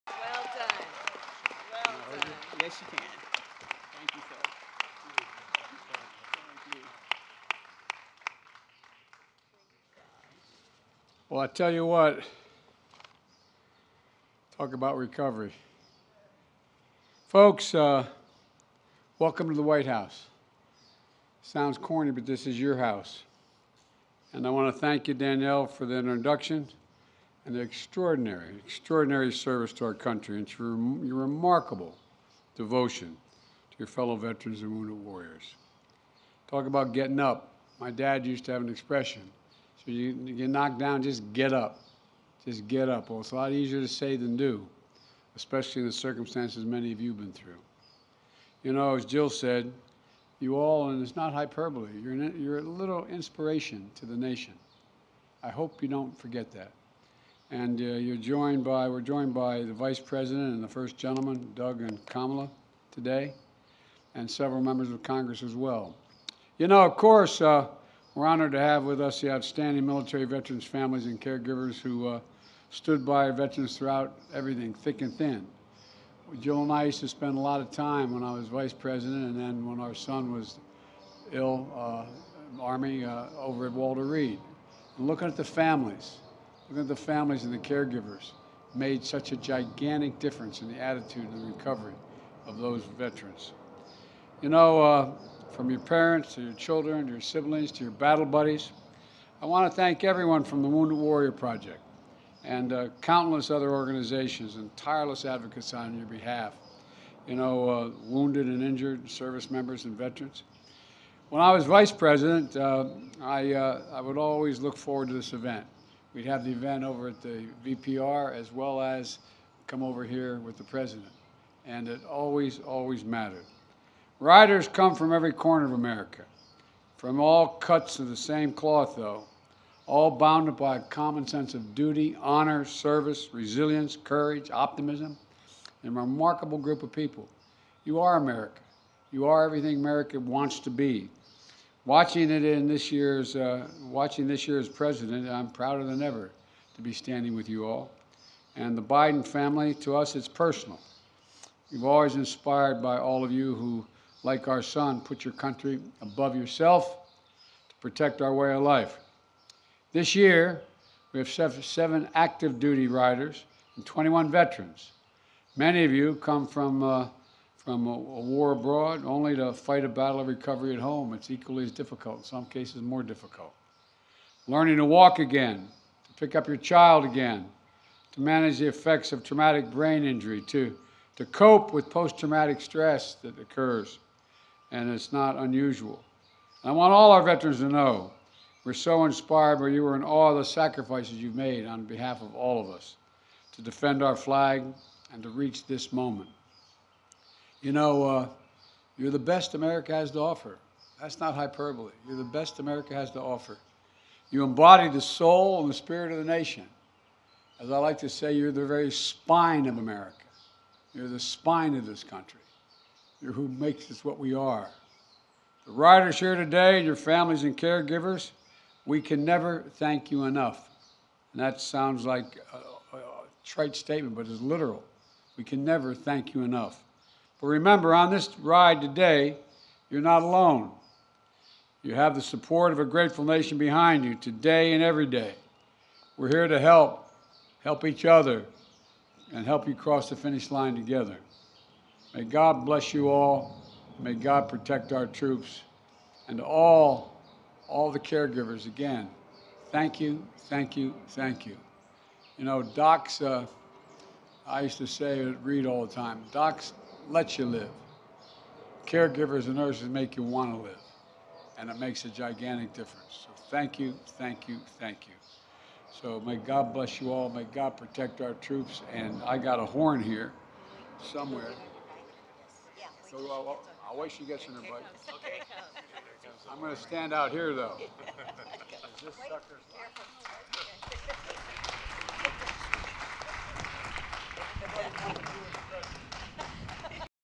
Remarks: Joe Biden Welcomes Wounded Warriors to the Annual Soldier Ride - June 23, 2022
Joe Biden welcomes the Wounded Warriors to the White House to start the annual Soldier Ride on June 23, 2022. Uploaded to YouTube for archival purposes by Factba.se